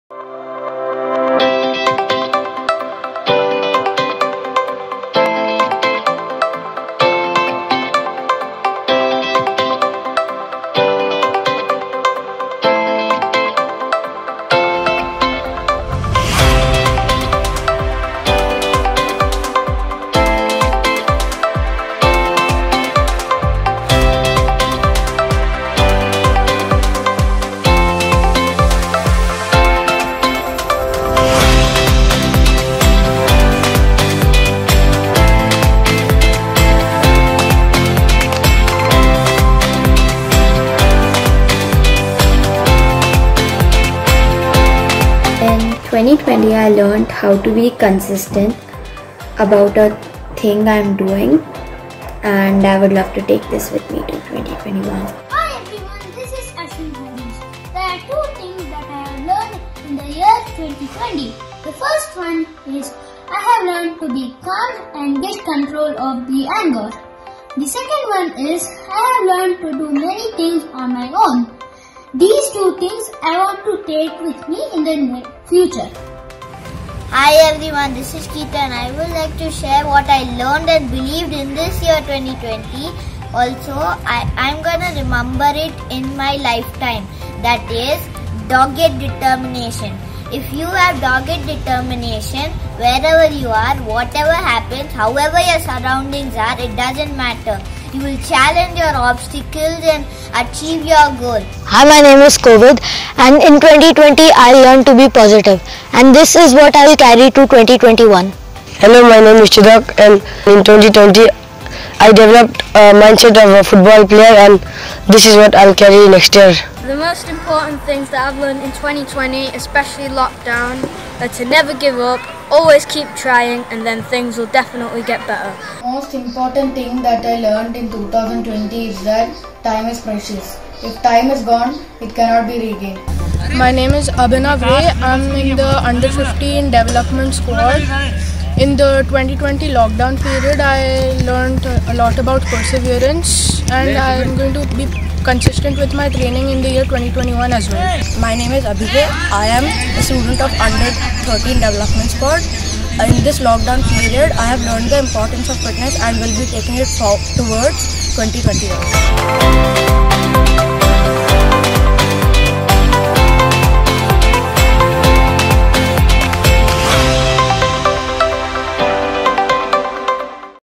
Happy New Year 2021
As we welcome 2021, our kids remind us that it is important to look back at our learnings and take them forward in the new year. The entire BBFSFamily wishes you a very happy new year. Stay safe and keep playing 🤩⚽ BBFSYes BBFSKids Happynewyear #2021